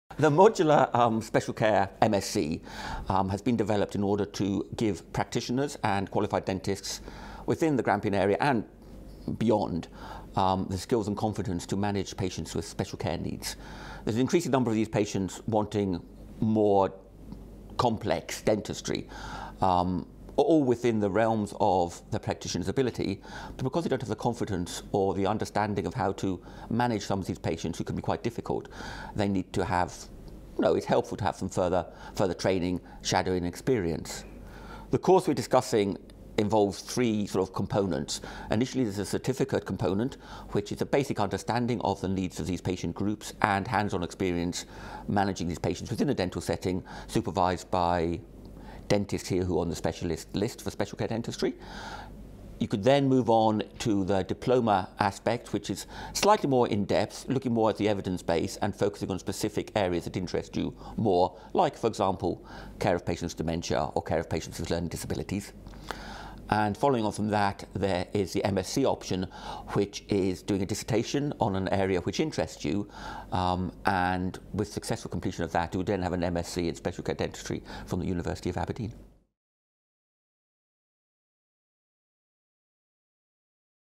Special Care Dentistry
This programme is aimed at graduated dentists who are looking to further their knowledge and experience of special care dentistry. The programme is designed to furnish dentists with a basic understanding of special care dentistry and the concept of patient centred care as well as provide guidance to develop critical thinking, care planning and delivery from evidenced sources.

Participants will initially gain a background knowledge in research / service planning as well as conditions and disabilities and get experience treating patients with special care needs in a supported environment.